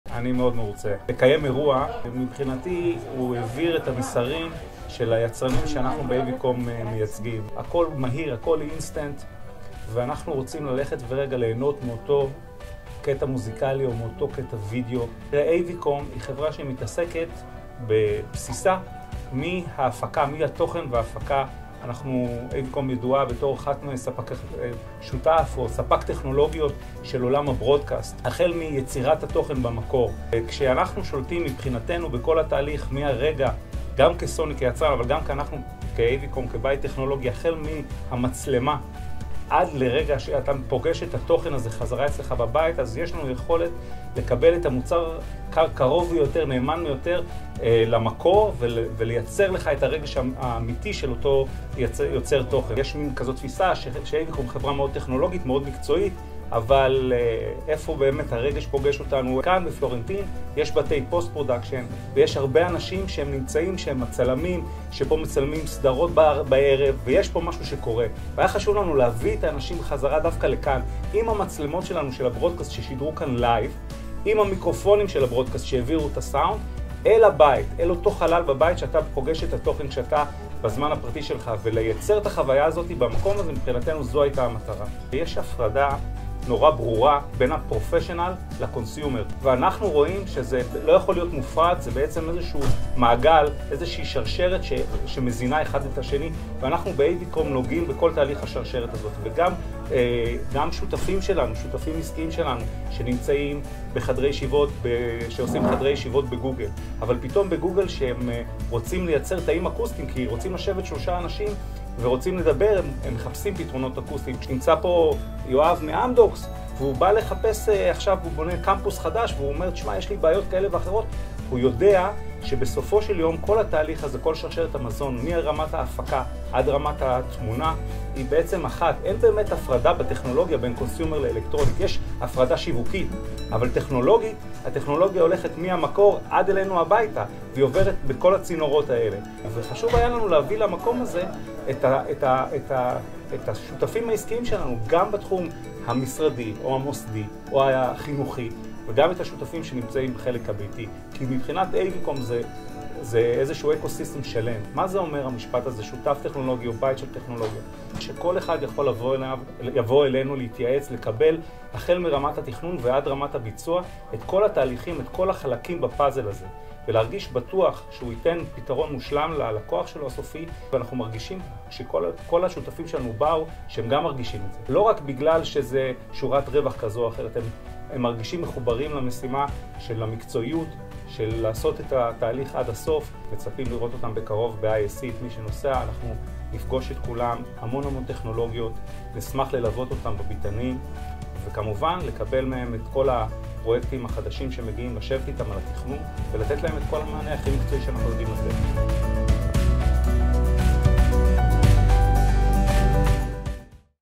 הברנג'ה נפגשה באירוע בוטיקי שערכה איויקום יחד עם שותפתה סוני. יואב גן מור מסכם
יום חמישי בערב נובמבר עם מזג אוויר שנראה שהוזמן גם הוא על ידי AVCOM. אנחנו מגיעים לאירוע ב-DHA שכולו שותפויות וטכנולוגיות אולם הדגש הוא על האנשים, ובאוויר מורגש ה-DNA של AVCOM, הממשק האנושי של היבואנית הטכנולוגית ניכר לעין ולאוזן, אותו תמהיל נכון של אנשים וטכנולוגיות בשפה וברמה גבוהה. לצד אלכוהול ותפריט מפנק פגשנו גם תצוגת תכלית משולבת